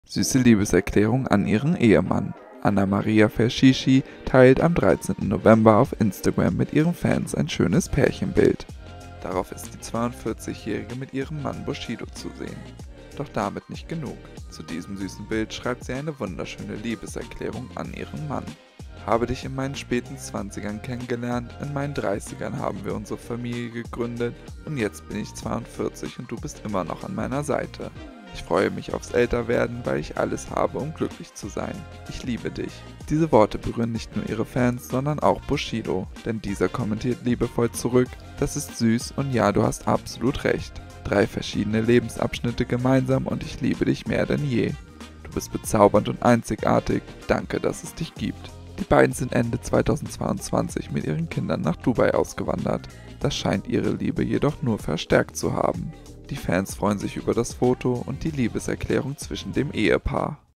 Anna-Maria Ferchichi macht Bushido eine Liebeserklärung
Anna-Maria Ferchichi teilte auf Instagram ein Pärchenfoto und machte damit eine Liebeserklärung an ihren Mann Bushido.

Impressum:
Burda Forward
St.-Martin-Straße 66
81541 München